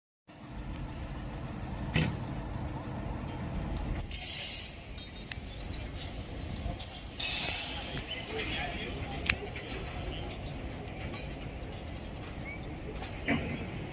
They Started to Film the New Star Wars in Valencia Spain
They're filming the latest Star Wars movie at the City of Arts and Sciences in Valencia: